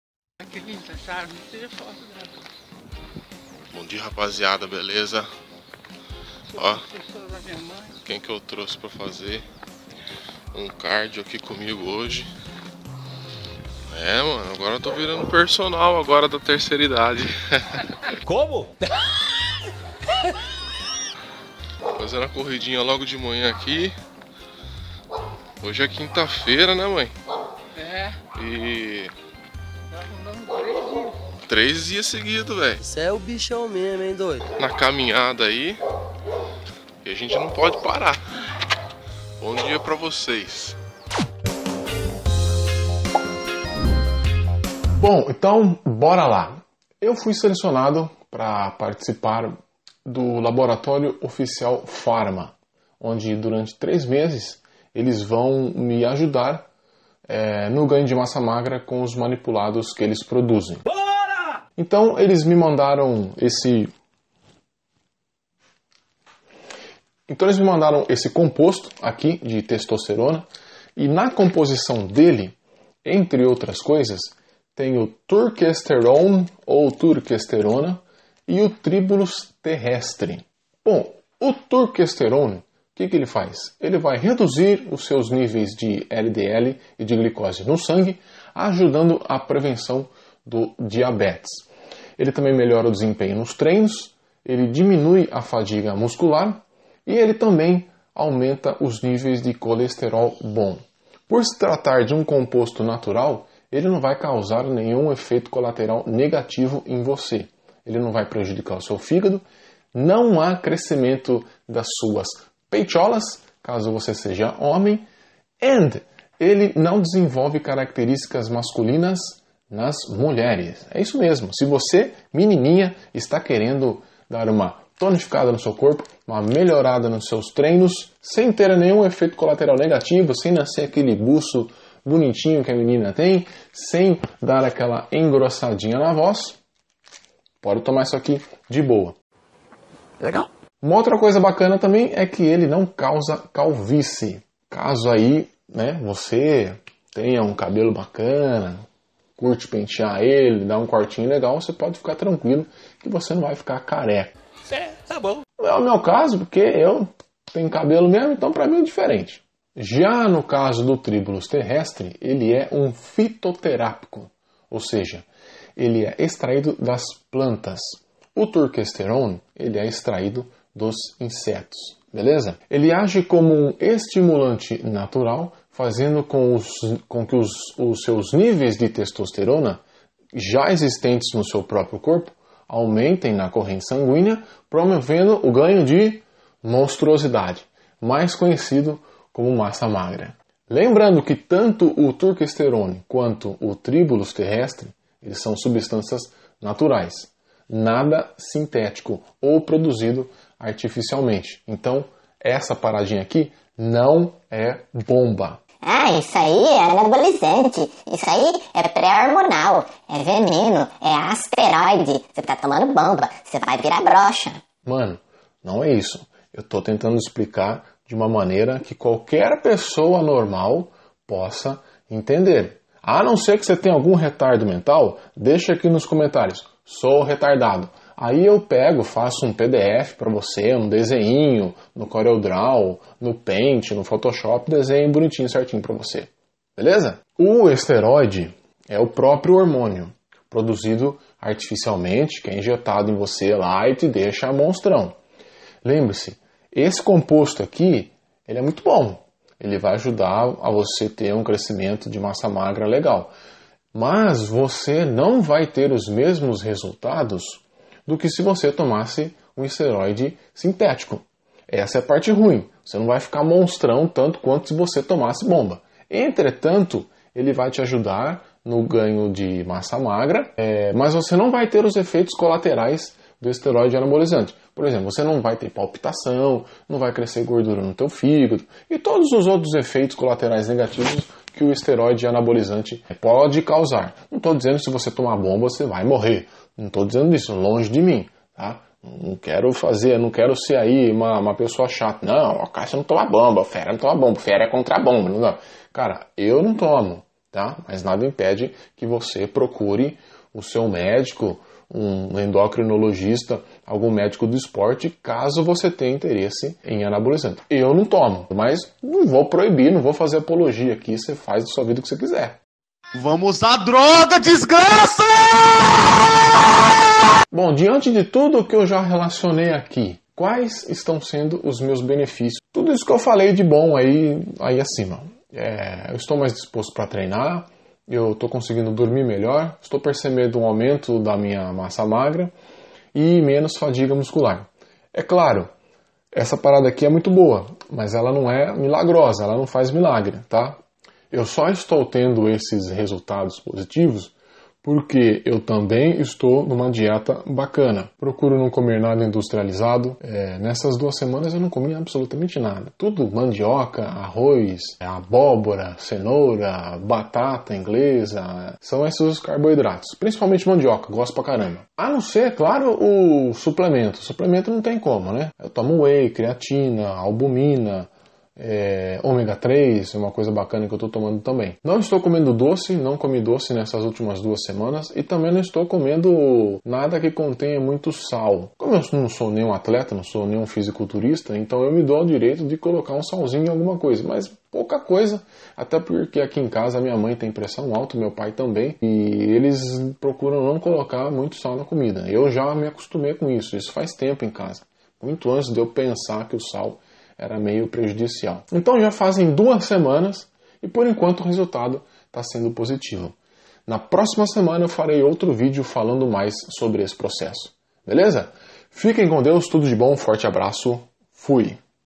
TURKESTERONE E TRIBULUS TERRESTRIS da OFICIAL FARMA Meus resultados após 2 semanas
Fui selecionado a participar do Laboratório Oficial Farma, onde por 3 meses terei o apoio de produtos manipulados por eles. Acompanhe o resultado nesse e nos próximos vídeos. Abraço Parte I